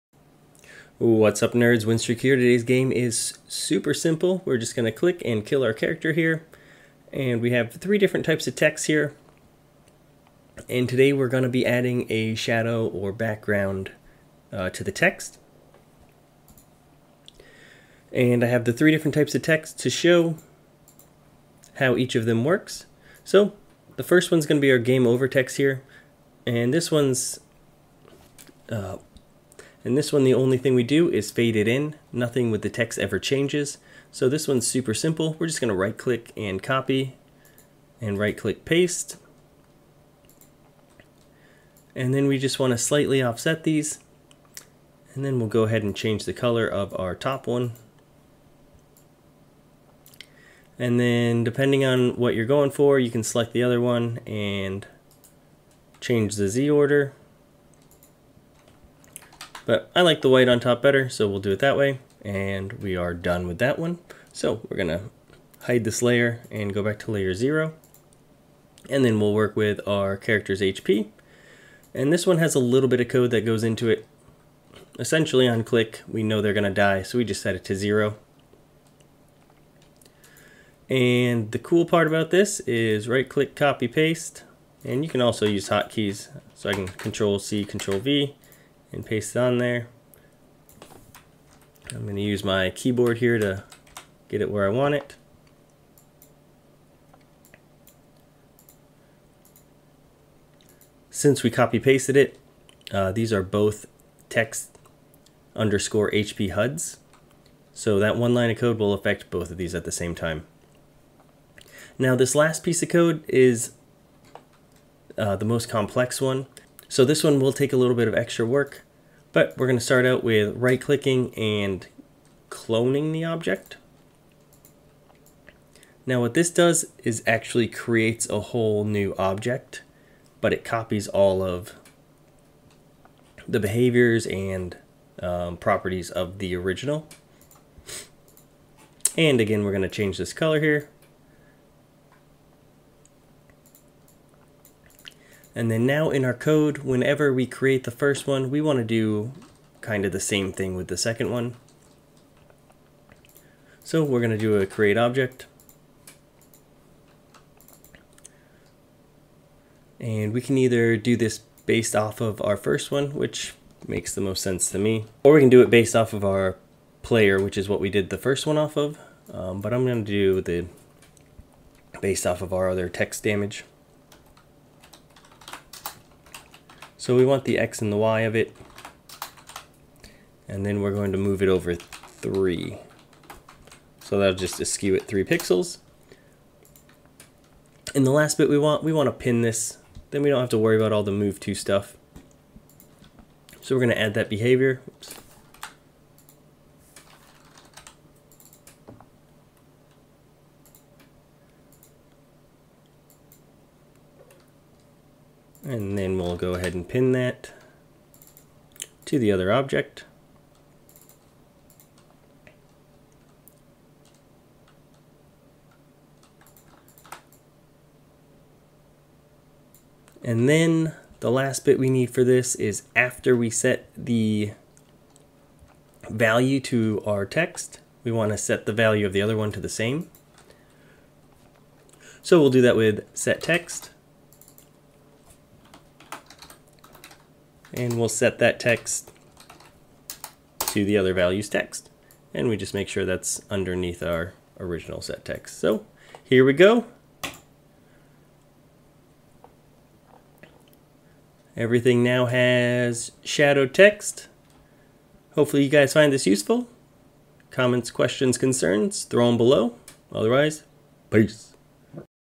Add Text Shadow in Game - Construct 3 Tutorial
How To Make your text look better by giving it more depth. Easily add text shadows to all your in game texts.